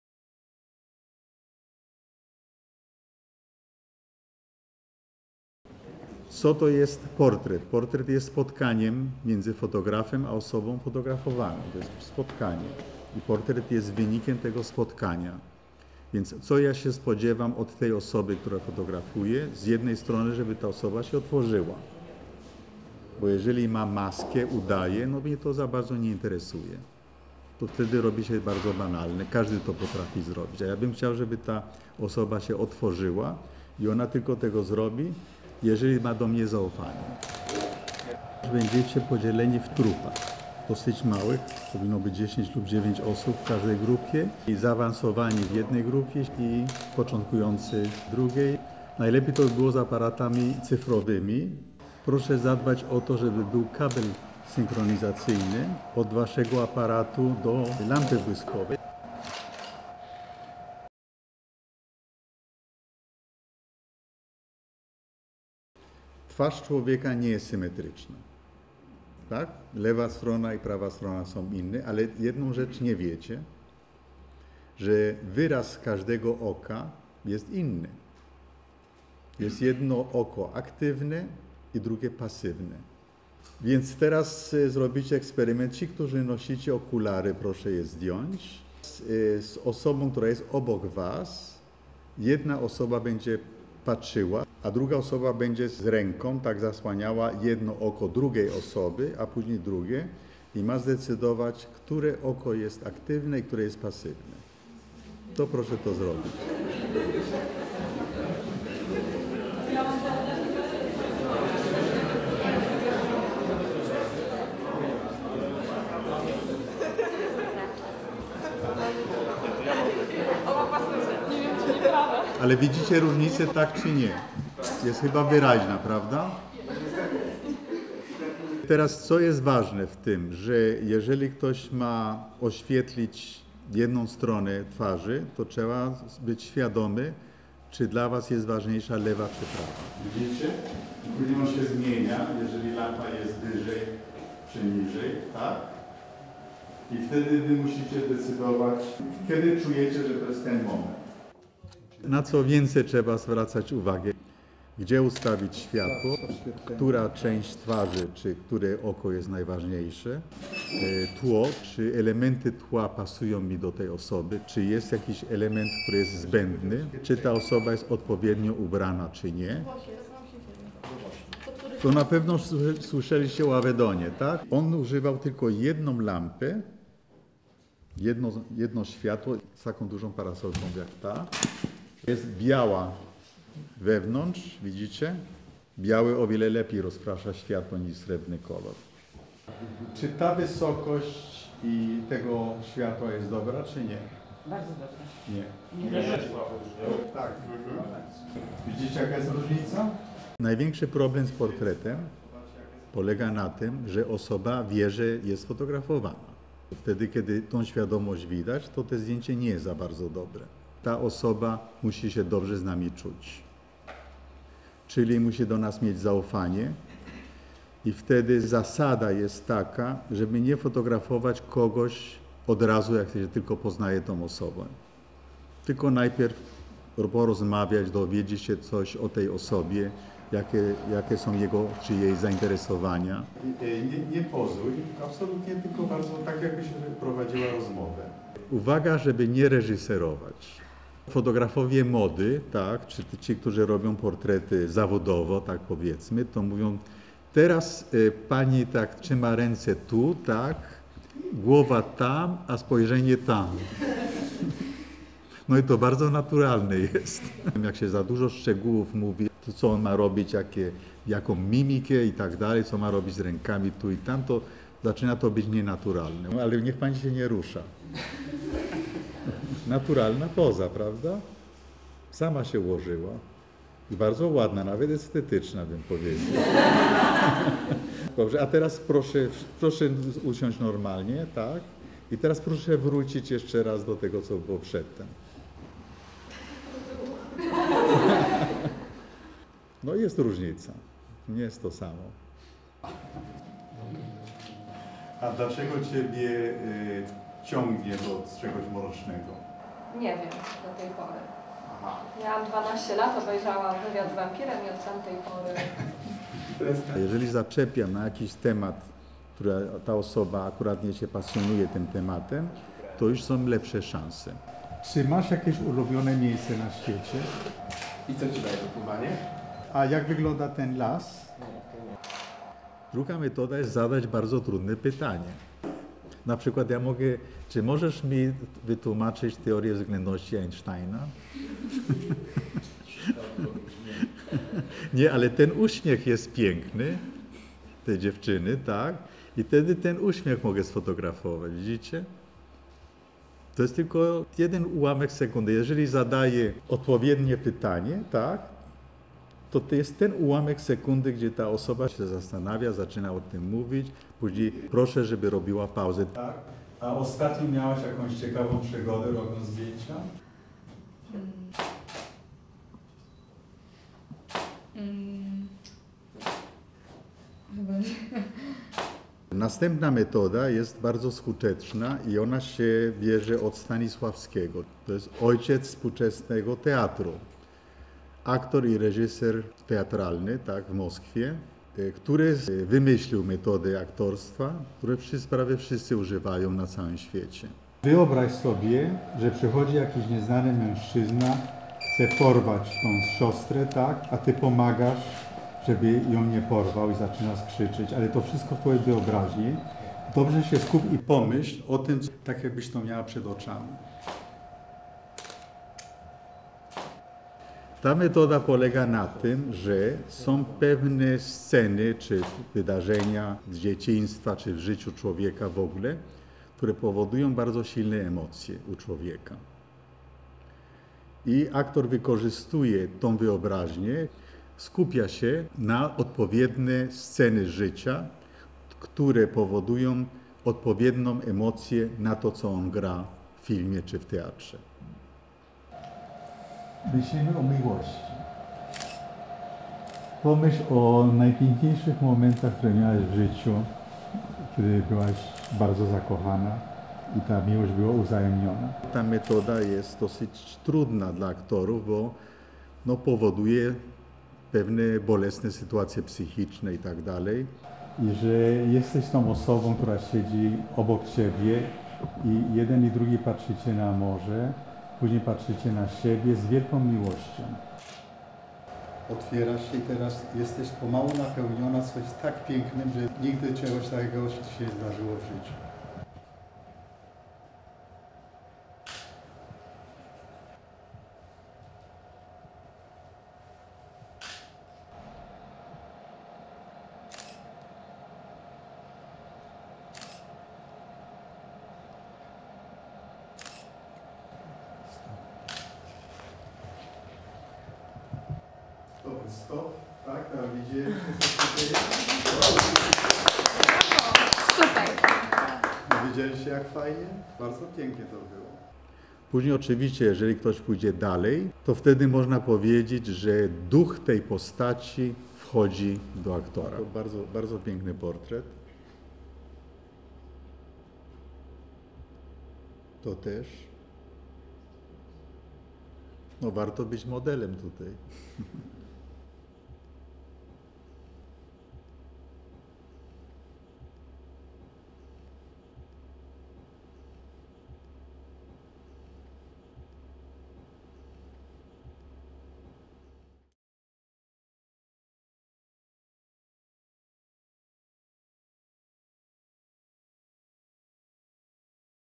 Marian Schmidt: Warsztat mistrzowski portretu fotograficznego.
Warsztaty mistrzowskie "Portret i fotografia ekspresyjna", prowadzi dr hab. Marian Schmidt, dyrektor Warszawskiej Szkoły Fotografii.  Master workshop in portrait photography. Film by Arthur Schmidt